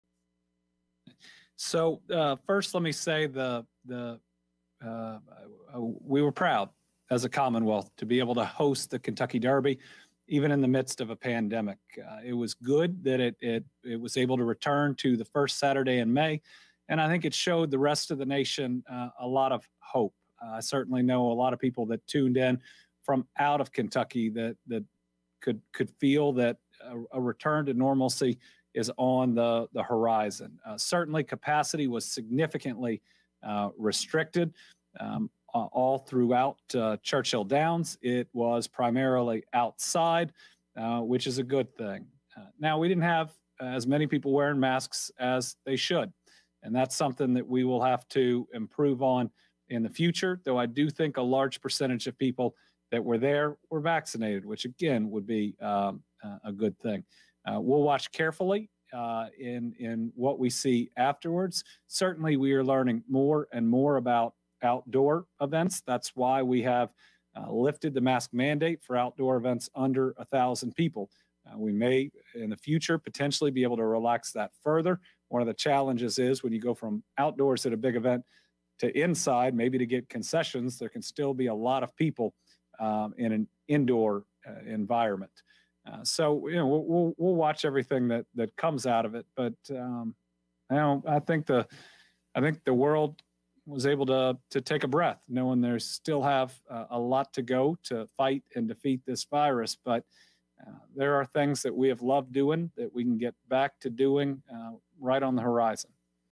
Gov. Andy Beshear discusses mask wearing during Kentucky Derby
The governor shares his thoughts after many were seen at the Kentucky Derby not wearing masks, despite a state mandate on venues with more than 1,000 people.